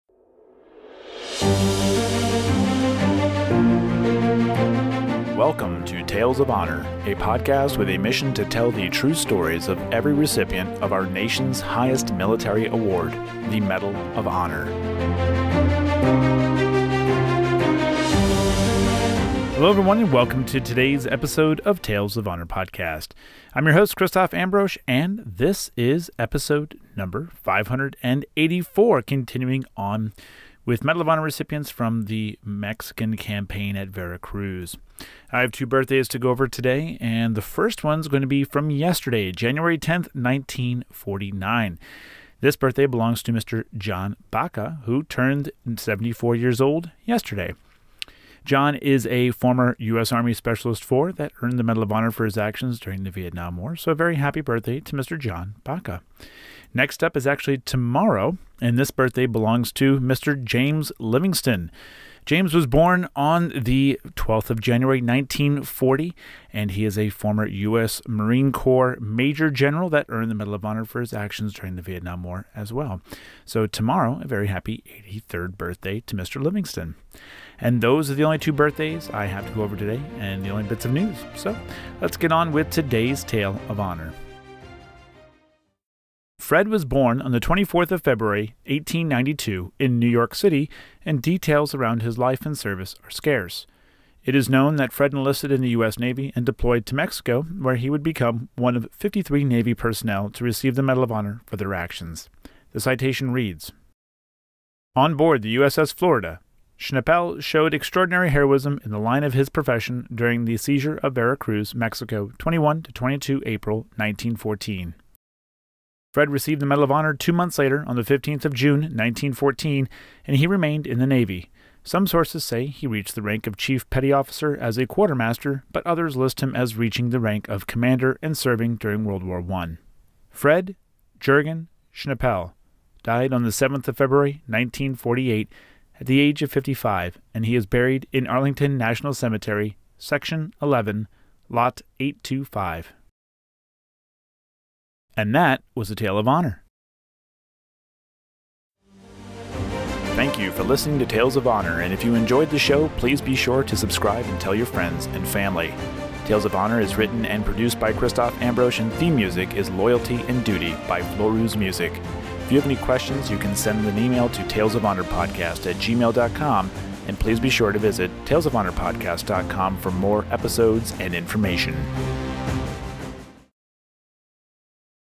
584. Fred J Schnepel - Medal of Honor Recipient
On episode five hundred and eighty-four, the story of Fred J Schnepel is told and episodes for all of January and February will be recipients from the Mexican Campaign at Veracruz.